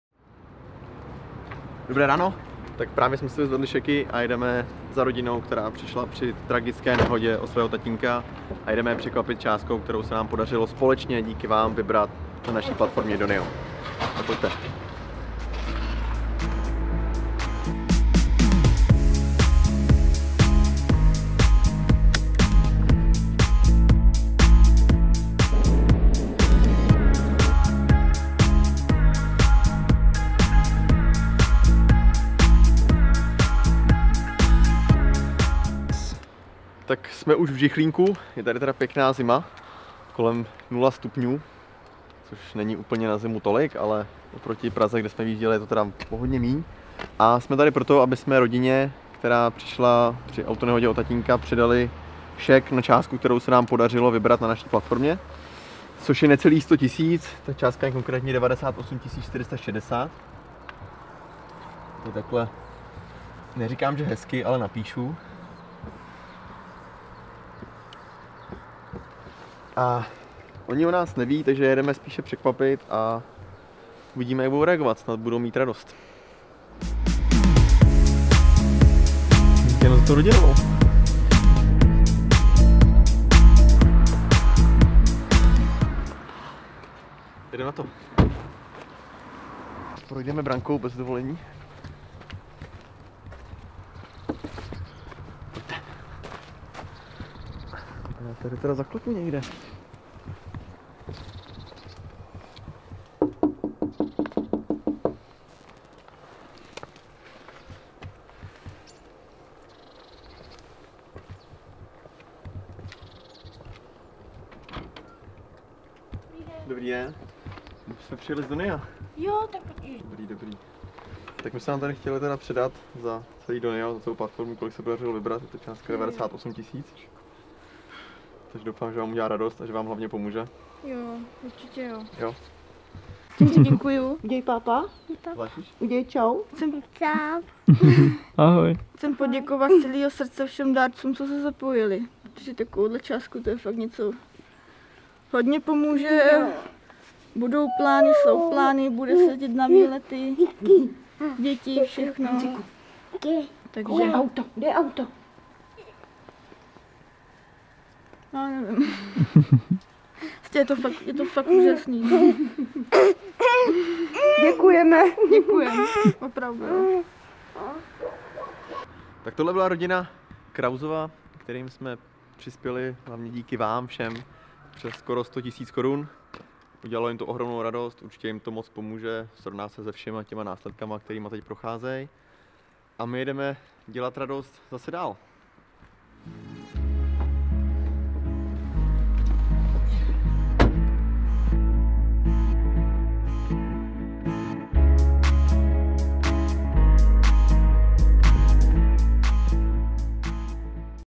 Pomohli jste rodině, která přišla o tátu 🧡
Maminka Dana a její dvě malé děti ležely po dopravní nehodě delší čas v nemocnici. Po probuzení zjistily, že přišly o tatínka Honzu. Honza byl nejenom milující táta a partnera, ale také živitel rodiny. A tak musela Dana s dětmi opustit byt, ve kterém žili, protože si z mateřské nemohla dovolit platit podnájem. Teď bydlí u babičky, v maličkatém zchátralém domě, a otáčí každou korunu. Děkujeme, že jste rodině pomohli, aby se v tak těžkém období mohla soustředit i na něco jiného, než jsou jen myšlenky na nelehkou finanční situaci. Krausovým jsme předali šek na částku 98 460 korun. Nic z toho by ale nebylo možné bez vás. DĚKUJEME!